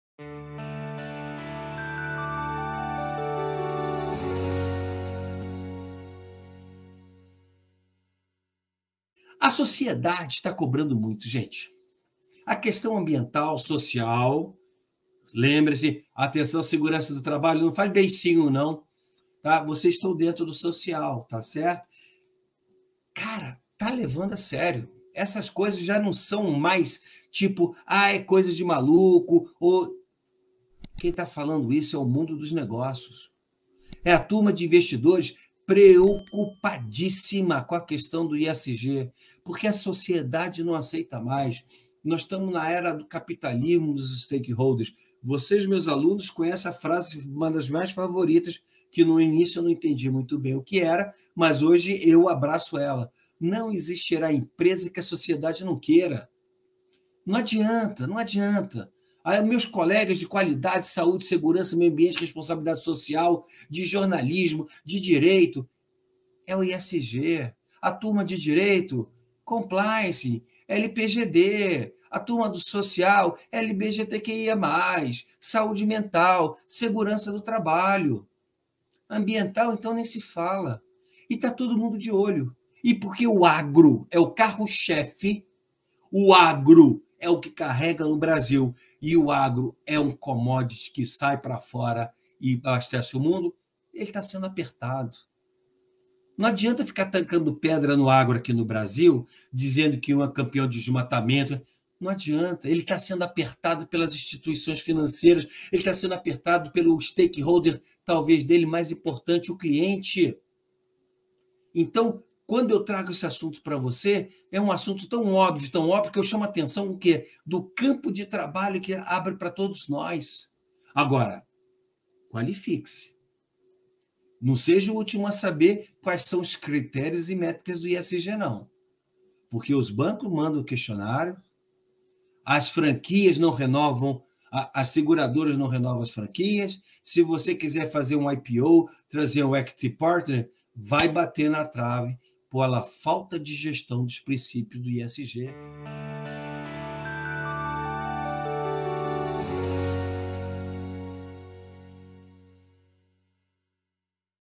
ESG no AGRO
O agronegócio brasileiro tem uma receita anual de cerca de R $1 trilhão. 💰

A diversidade de solo e clima tornou o Brasil conhecido como a terra onde tudo que se planta dá, mas o mais importante é que o crescimento seja acompanhado da profissionalização da gestão rural. ⬅️

➡ ️ A questão ESG já é destaque na maioria dos setores da economia do Brasil e do mundo e o setor do agronegócio, devido a sua importância na economia, não poderia ficar de fora do movimento.

As empresas que adotam estratégias e modelos de negócios alinhados às melhores práticas ESG se diferenciarão no mercado e estabelecerão as bases para seu crescimento e sustentabilidade. ♻️

👉 Quanto mais cedo a sua empresa se preparar para enfrentar esse desafio, maiores serão suas chances de sucesso.

Nosso país é uma potência agroambiental. Atrelar o ESG ao agro é ser protagonista de uma mudança que já está acontecendo. 🤷‍♂️

Clique no vídeo para entender mais. 👍